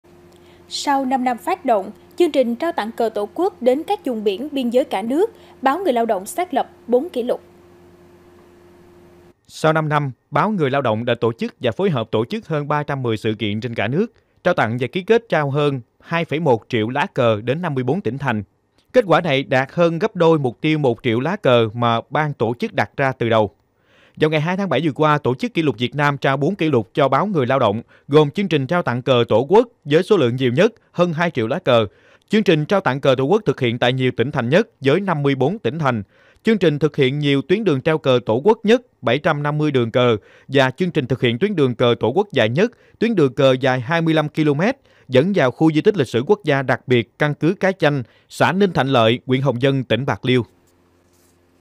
Chương trình Tự hào cờ Tổ quốc xác lập 4 kỷ lục | Cần Thơ TV
Sau 5 năm phát động chương trình trao tặng cờ Tổ quốc đến các vùng biển, biên giới trên cả nước, Báo Người Lao Động đã xác lập 4 kỷ lục.